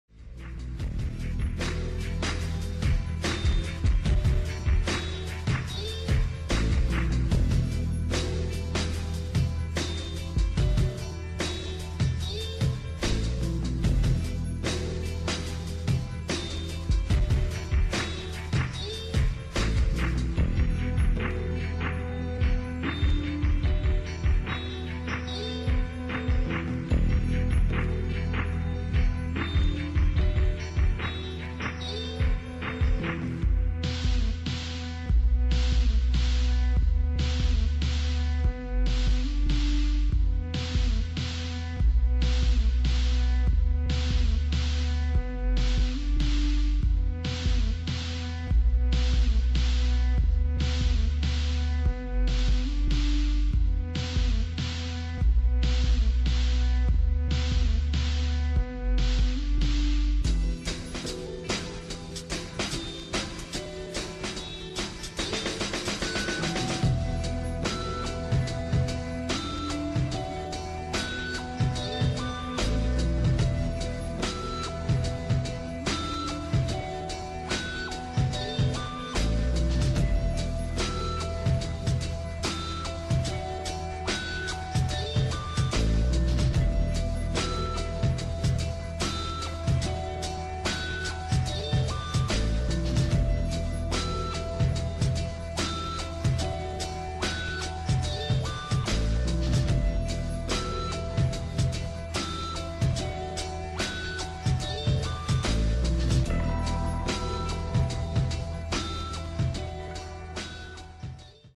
2 incher with cold balls MICRO RIPPIN - DVR FOOTAGE -
Turtle mode indeed!  - It's fuckin cold out here but I just built a 2-inch micro..  This is the second flight.. no real adjustment to rates, just pid tuning so far.

Bit unorthodox like most of my builds but I love it, rips hard -

140.3g w/battery Turnigy 700mAh 11.3 @ 12.8 65c, flat cells, low cog
AirBlade Eclair 3K Carbon Fiber Frame Kit V2 (2.5in version)
AirBlade AB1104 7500kV 2S-3S Brushless Racing Motors
20A 4in1 ESC - (not sure of the brand)
Micro F3 Flight Controller on BF3.2, DSHOT600, 8/2k
200mW AIO Cam w/2.1 lens and a dipole antenna.